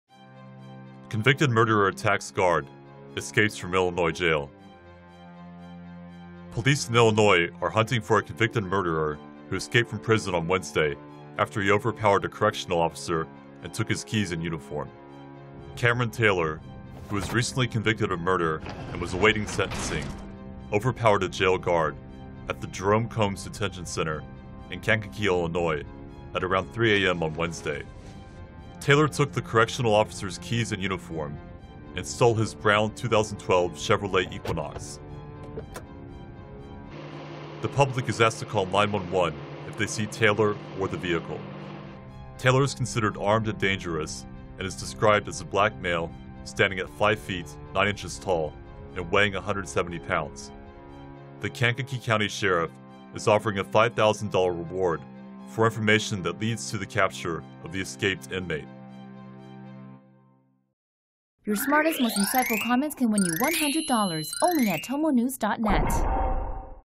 Murderer escapes prison by attacking guard, stealing his car, in Kankakee, Illinois
Police in Illinois are hunting for a convicted murderer who escaped from prison Wednesday after he overpowered a correctional officer and took his keys and uniform.

The inmate, identified as Kamron T. Taylor, 23, escaped from the Jerome Combs Detention Center in Kankakee, about 60 miles south of Chicago, at about 3 a.m. Taylor stole the correctional officer's Brown 2012 Chevrolet Equinox with Illinois license plate P506660, the Kankakee County Sheriff's Office said in a statement.

Taylor is considered armed and dangerous and is described as a black male standing 5 feet, 9 inches tall and weighing 170 pounds, according to the sheriff.

Taylor, who has a history of escape attempts, was convicted for the 2013 murder of 21-year-old Nelson Williams Jr. during an attempted robbery at Williams’ home in Kankakee. He faces a sentence of 45 years to life.

The Kankakee County Sheriff’s Office is offering a $5,000 reward for information that leads to the capture of the escaped inmate.